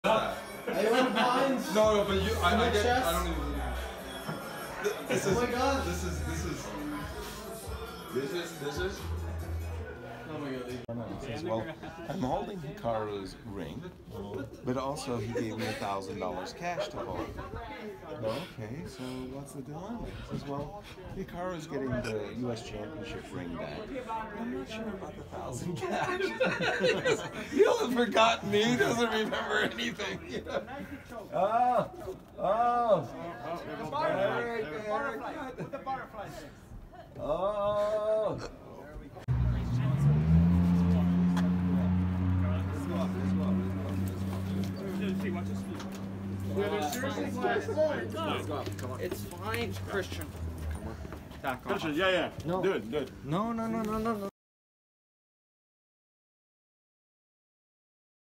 Eric Hansen and Hikaru Nakamura FULL FIGHT
Footage not initially leaked by me; a portion of this was posted one day earlier without my knowledge, and I uploaded to give all my footage. April 29, 2018, last night of the 2018 US Chess Champs. All my own footage.